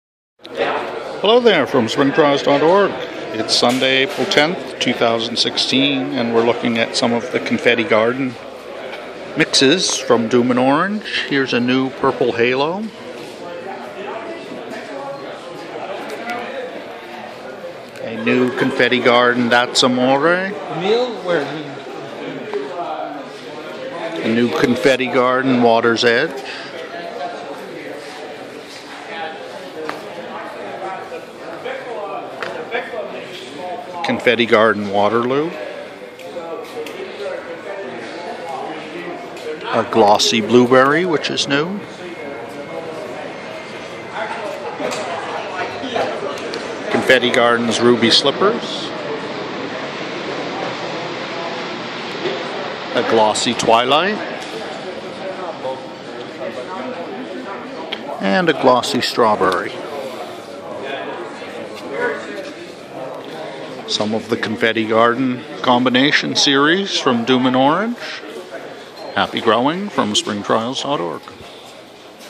Spring Trials 2016: Confettia Garden® Mixes II
From Dummen Orange, RedFox Confetti Garden Combinations, featuring: 'Purple Halo' (new), 'That's Amore' (new), 'Water's Edge' (new), 'Waterloo', 'Glossy Blueberry' (new), 'Ruby Slippers', 'Glossy Twilight' and 'Glossy Strawberry'.